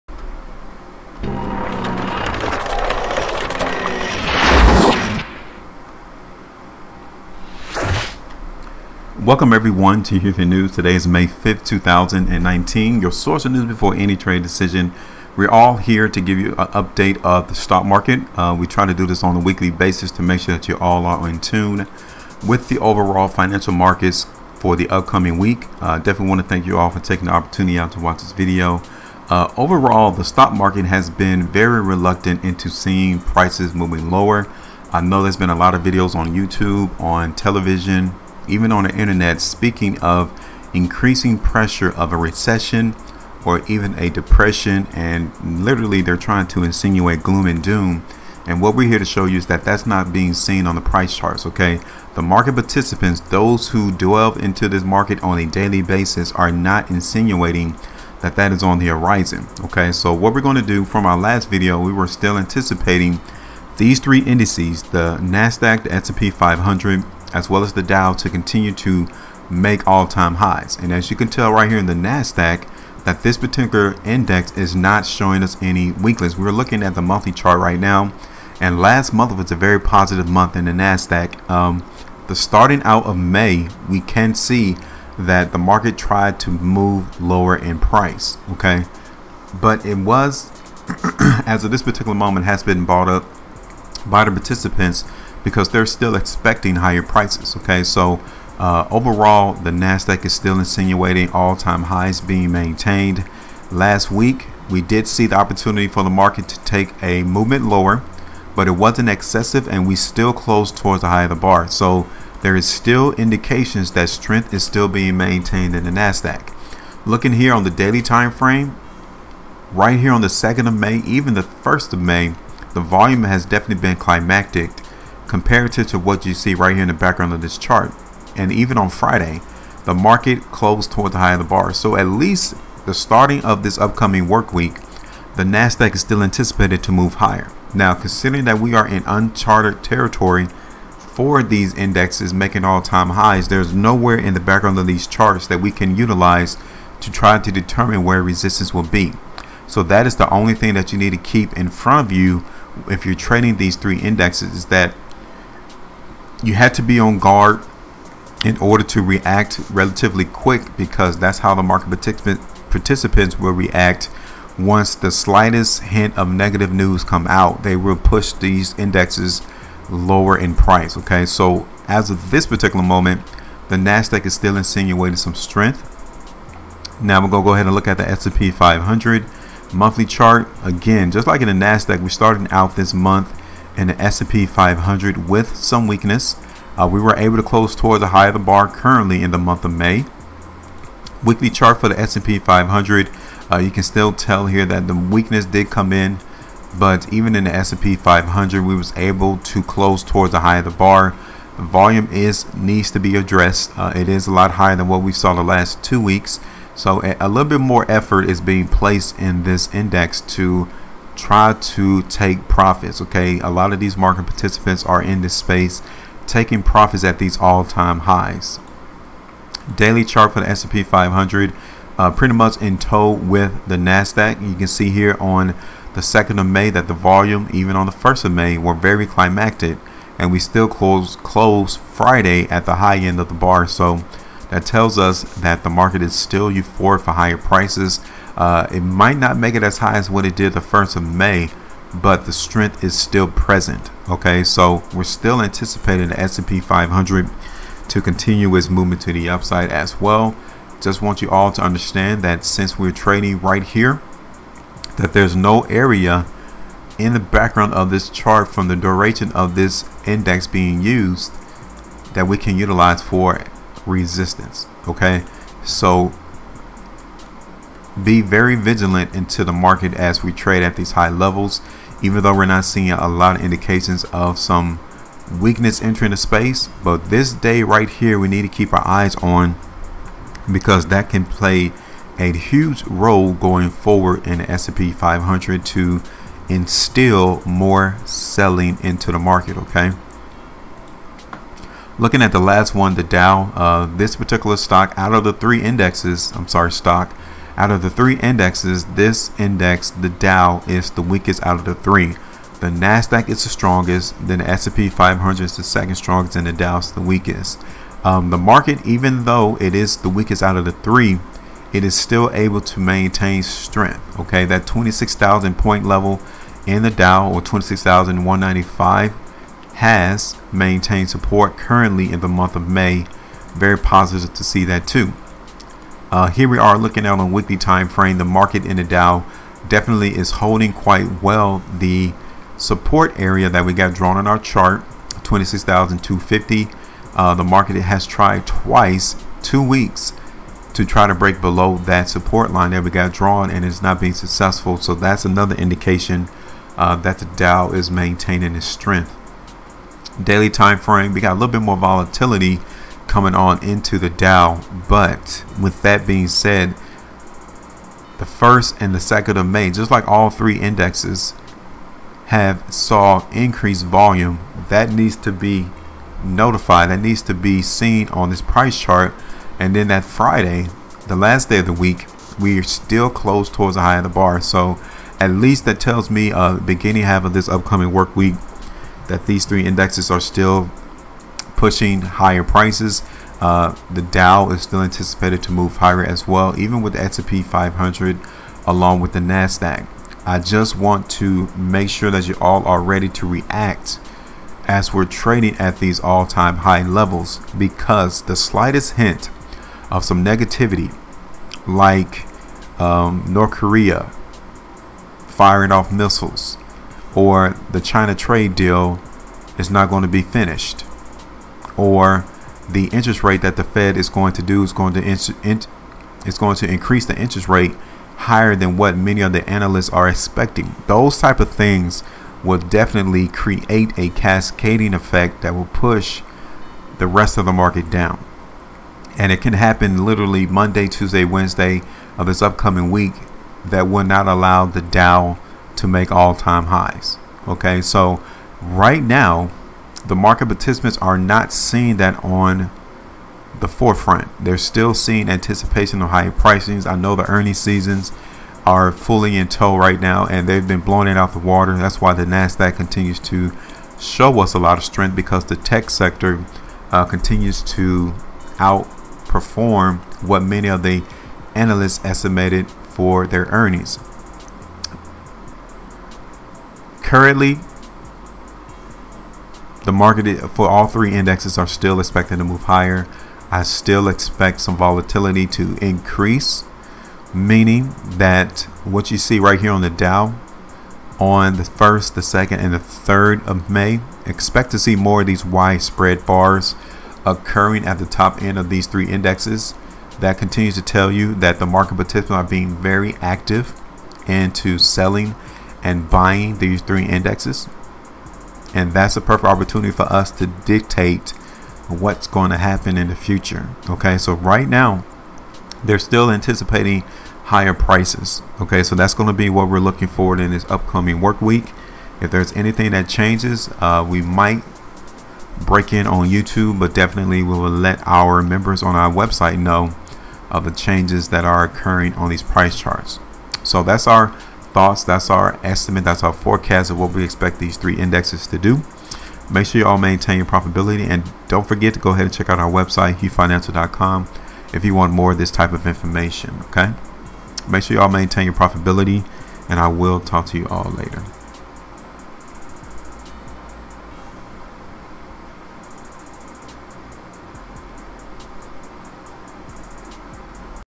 STOCK MARKET OUTLOOK | NOW IS THE TIME TO BUY?
Seeing the stock market make all time highs seemingly on a daily basis; would now be the time to buy the market?  We'll look at the price chart to see what it has to say.